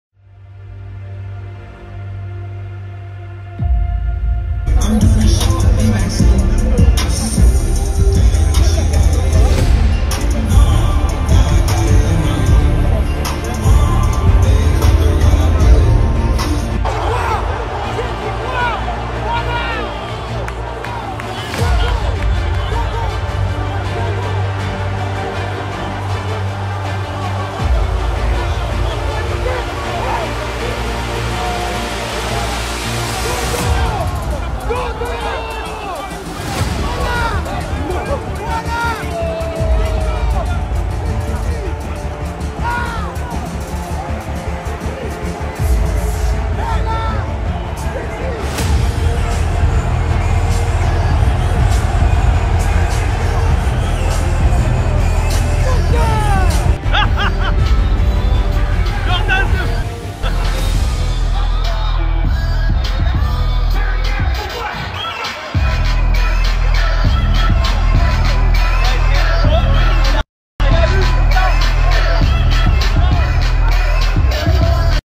MBAPPÉ CHOQUÉ PAR LE KO DE CÉDRIC DOUMBÉ (JORDAN ZEBO) AVEC DES PARISIENS APRÈS CLERMONT 0-0 PSG !
MBAPPÉ ET QUELQUES COÉQUIPIERS AU COMBAT ENTRE CÉDRIC DOUMBÉ ET JORDAN ZEBO ! (bonus) - FOOTBALL 2023

Salut à tous ! On se retrouve aujourd'hui pour une petite vidéo bonus, sans commentaire perso, sur la réaction de Mbappé au KO de Cédric Doumbé !

(FR/Français)